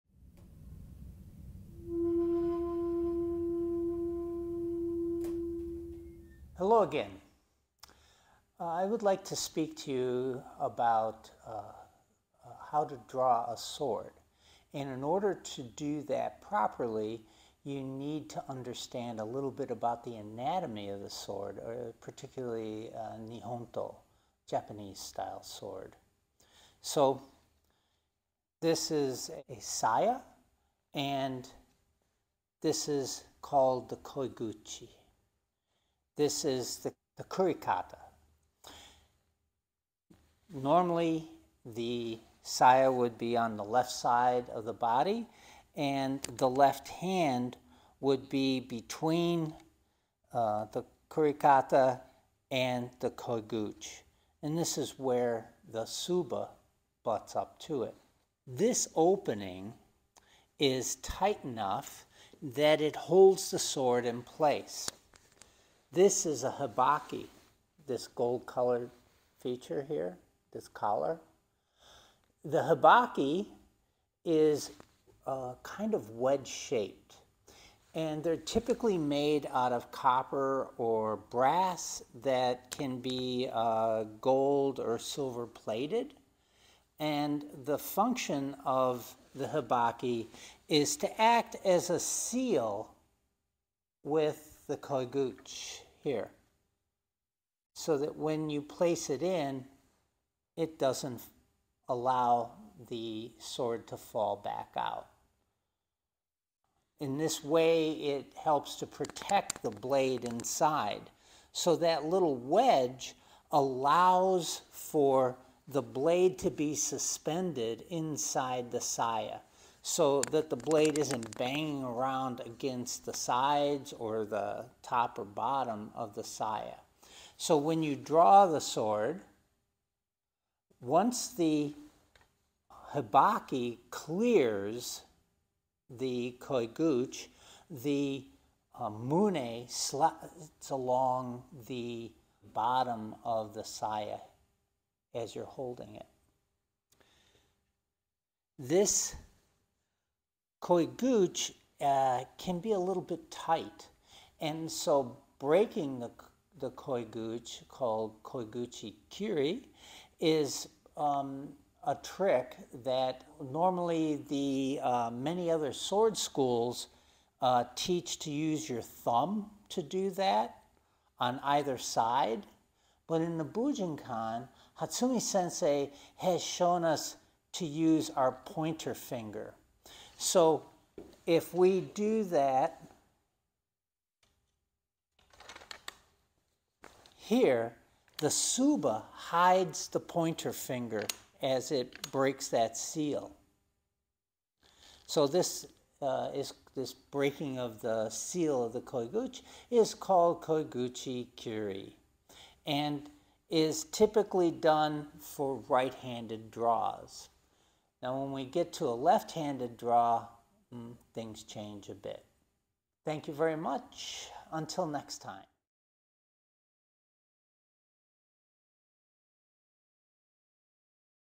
AZBujinkan - 38 - Anatomy Of The Nihontō
Examining some of the parts of the nihontō (Japanese sword) as they relate to drawing the sword.